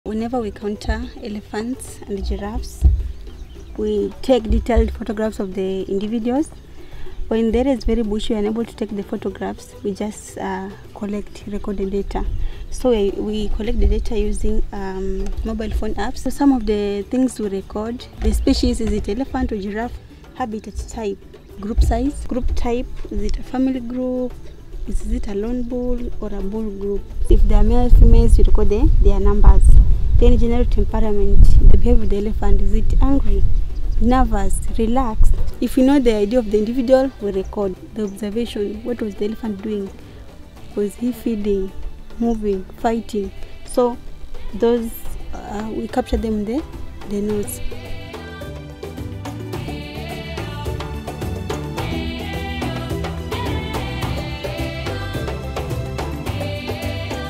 Elephant and giraffe monitoring in Meru National Park
Irene talks through how we monitor elephants and giraffes in the field. By doing this, we can build up a database of individuals living in Meru National Park, and understand the best way to conserve them.

This is just small way technology is used as an important part of conservation - from GPS collars and camera traps, to satellites!